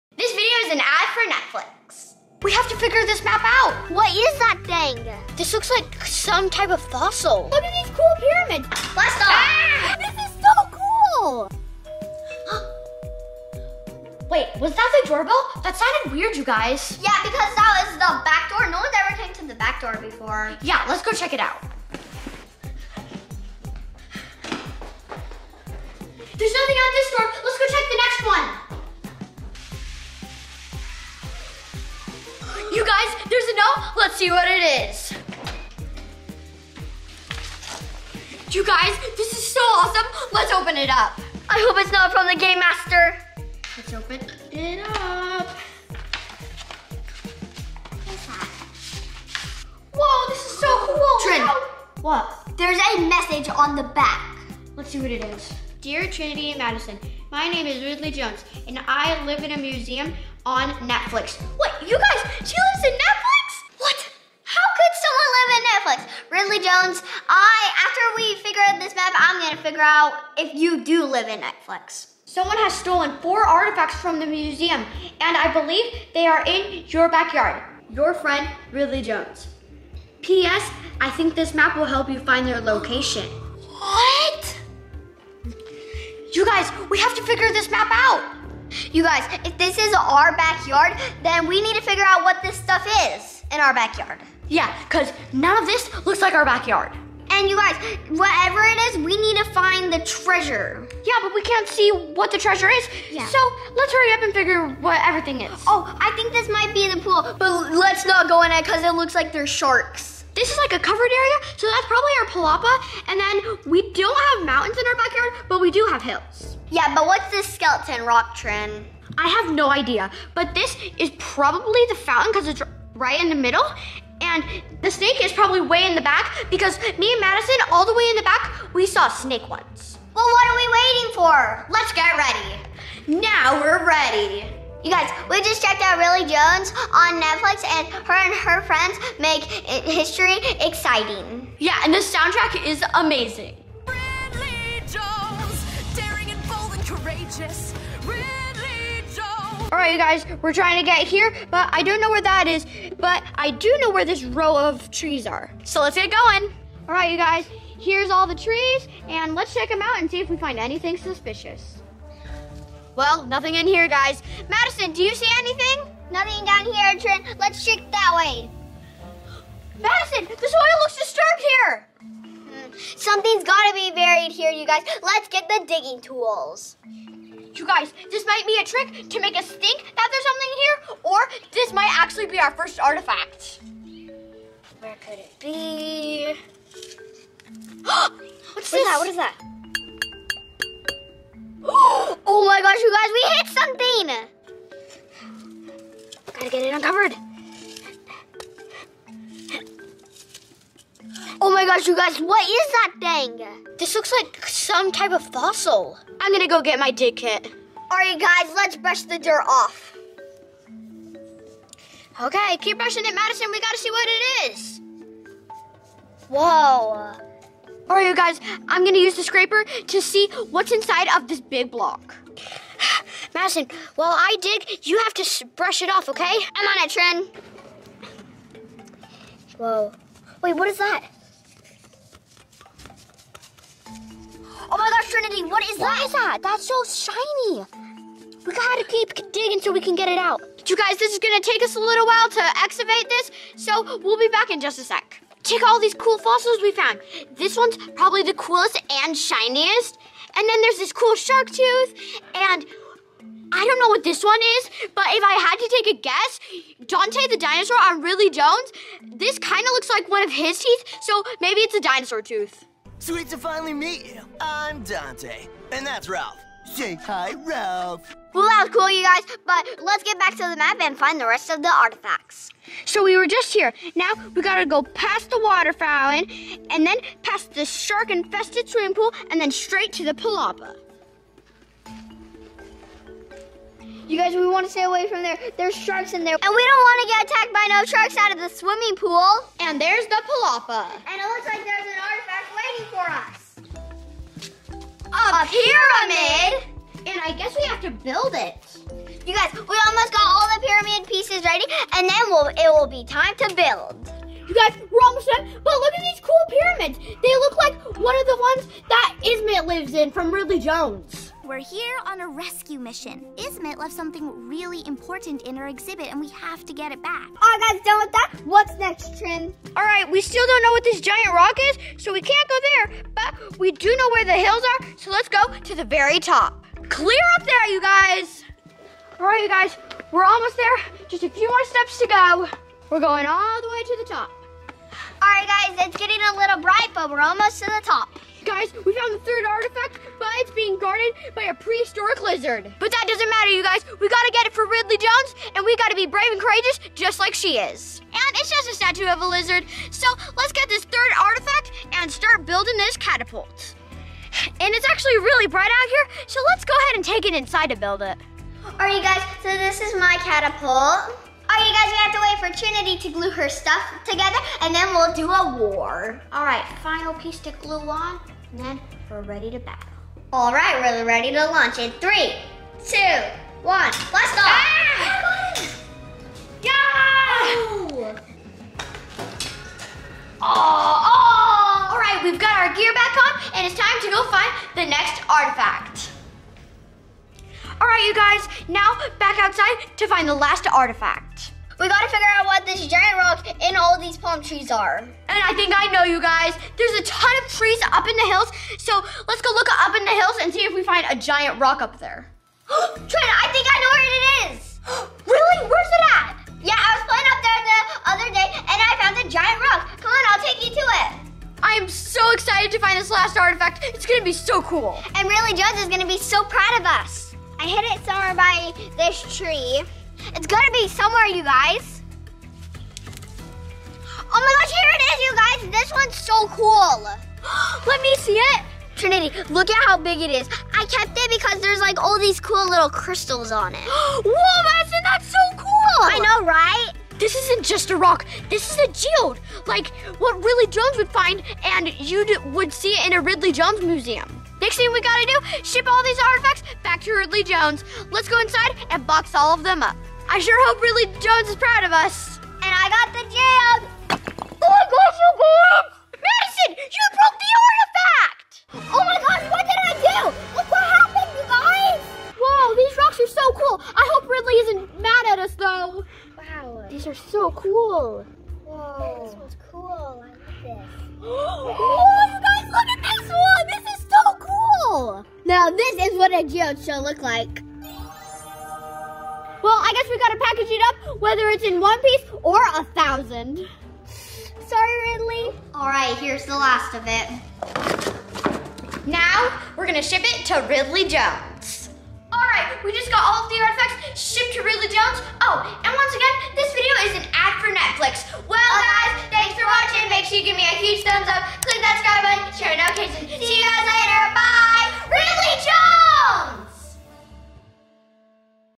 We Find Lost Artifacts in Our Backyard!!!
Scavenger Hunt for Lost Artifacts in Our Backyard!!!

Hunting For Lost Artifacts!!! Trinity and Madison have been asked to help Ridley Jones find her lost artifacts! With the help of the map, they're able to locate each artifact but there ends up being a big mess at the end!

Watch Ridley Jones on Netflix now!